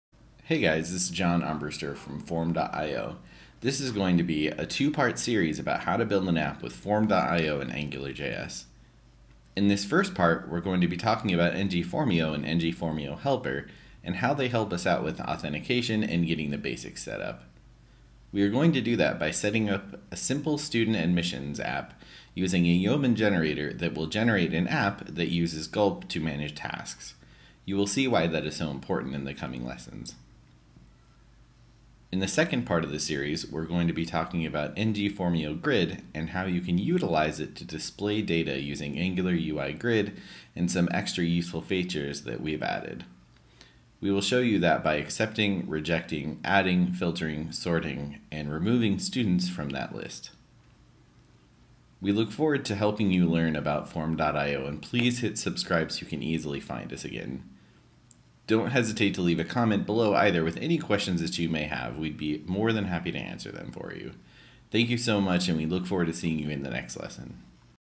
Form.io Developer Tutorial Part 1: Introduction
This is an introduction to a multi-part series about how to get going with Form.io. We are going to put together a student admissions app with Form.io forms.